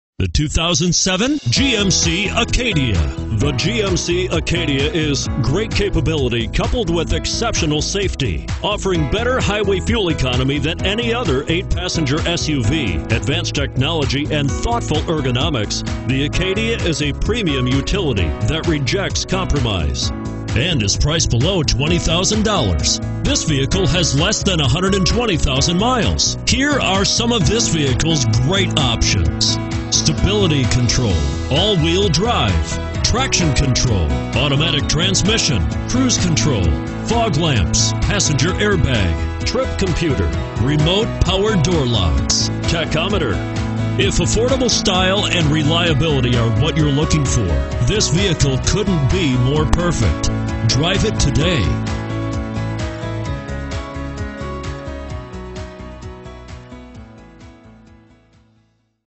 2007 GMC Acadia Carmel NY GL0505
2007 GMC Acadia SLT - $17055

Looking for the right car? Today could be your lucky day. With 118210 miles, this Blue, 2007 GMC Acadia equipped with Automatic transmission could be yours. Request more information and set up a test drive right away.